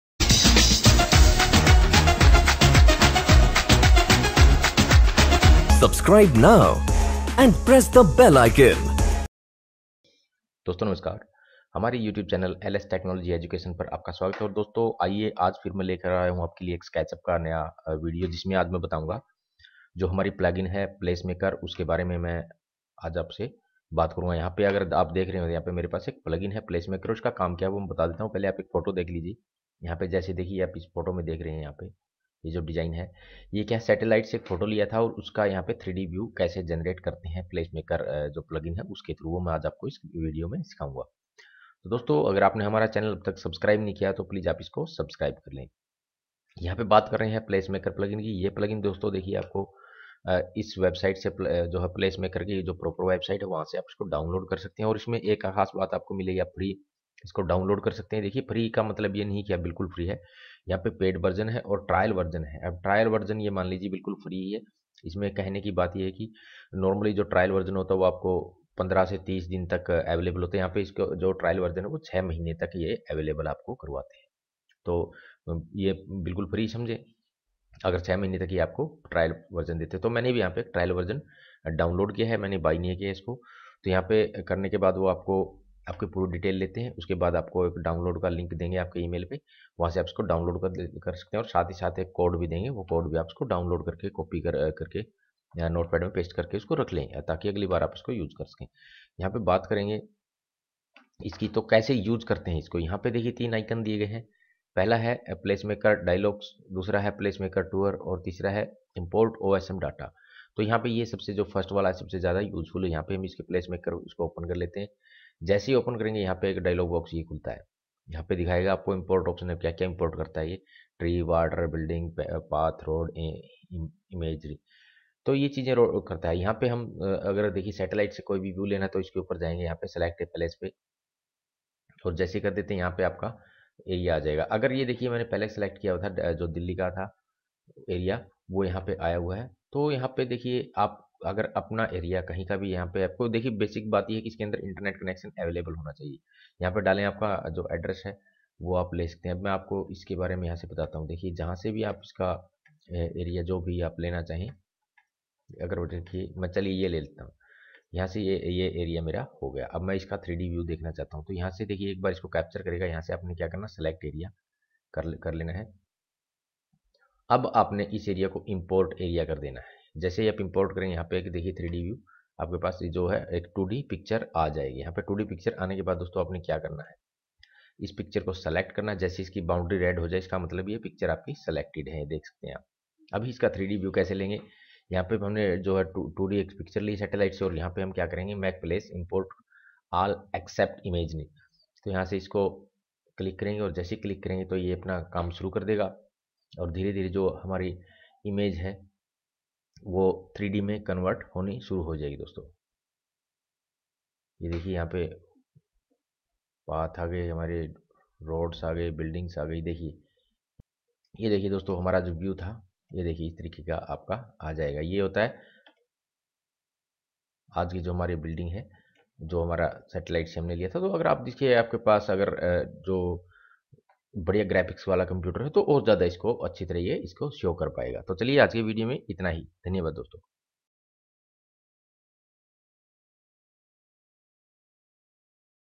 Fast 3D model a city in SketchUp with Place maker plugin. किसी भी शहर का 3D मॉडल कैसे बनाएं !
दोस्तों इस वीडियो में आपके लिए बहुत जानकारी है।
Hello friend here i added some of Sketchup tutorials in Hindi for that who they cant learn in English.
I hope you will like the video as i have the knowledge of SketchUp i providing you. thank you so much.